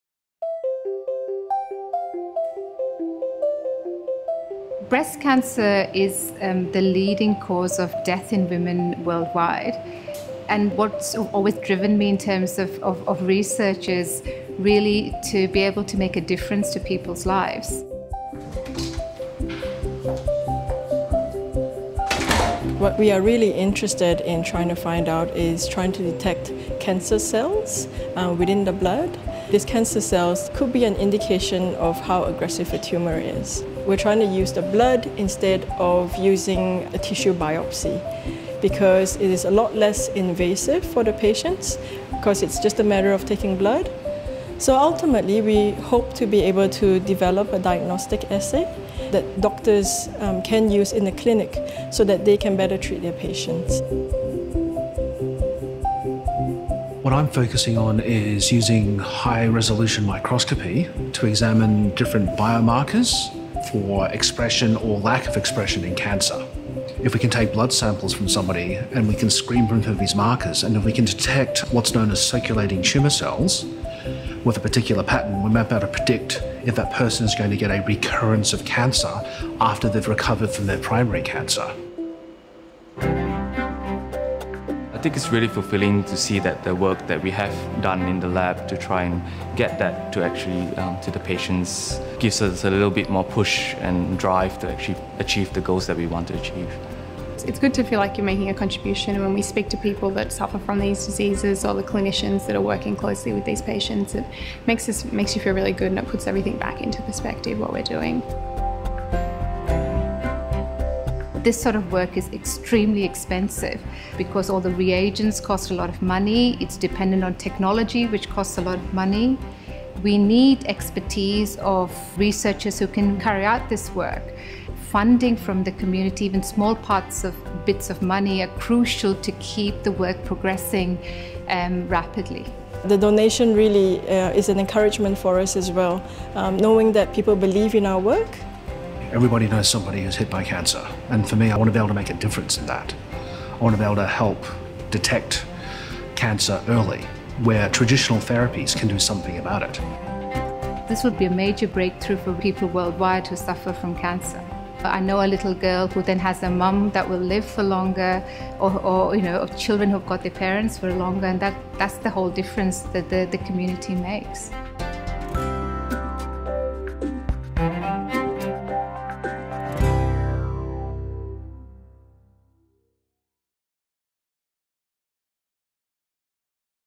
How philanthropy is helping prevent cancer recurrence at the University of Canberra
The UC Cancer Research Fund supports the activities of Dr Sudha Rao and her team, who are tirelessly working to prevent cancer recurrence and improve treatment. Metastasis and recurrence of cancer are associated with over 90% of cancer deaths.